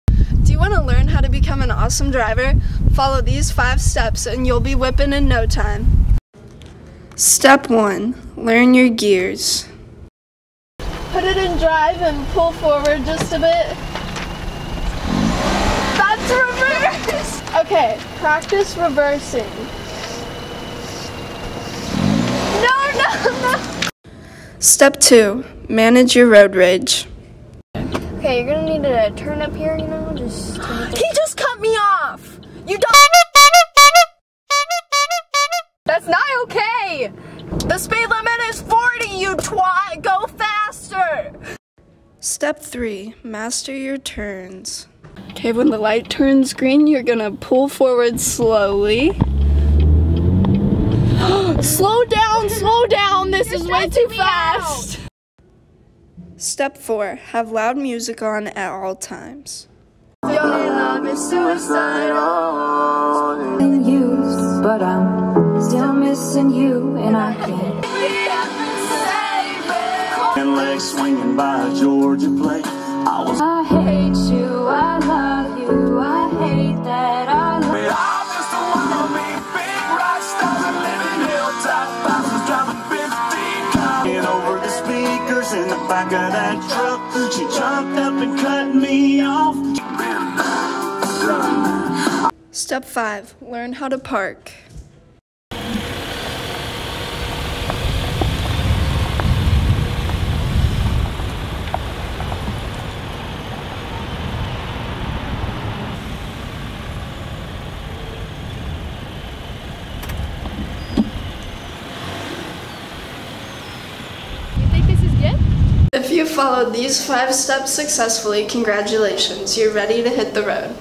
The Five: how to drive
In this video we give you five steps on how to be an awesome driver. This is a comedic skit meant to make you laugh. These steps are not to be taken seriously. We do not recommend following them. Enjoy the video!